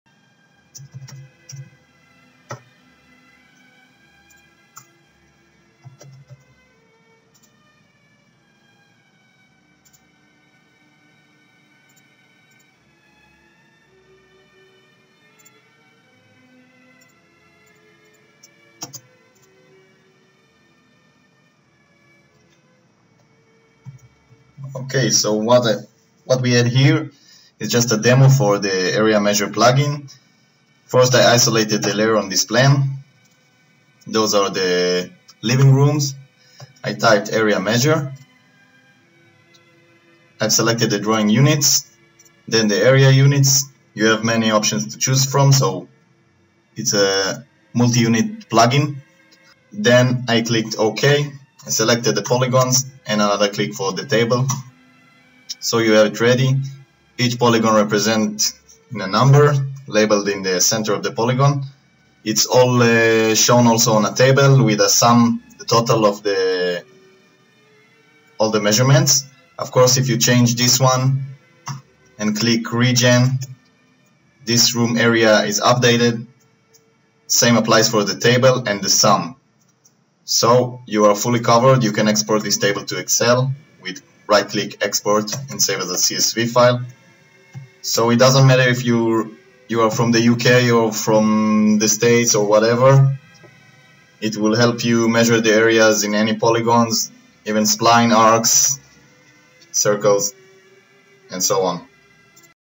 Area Measure Polygon Autocad
Automatic Measurement Of Polygon Areas in AutoCAD®
Output In A Choice Of Metric or Imperial Units*
Automatic Numbering & Labeling Of Polygons
Automatic Table Of Selected Polygons Including a Grand Total Area.
Lifetime License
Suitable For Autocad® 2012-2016
*mm², cm², m², km², Dunam, Ares, Hectares, in², ft², yrd², Acres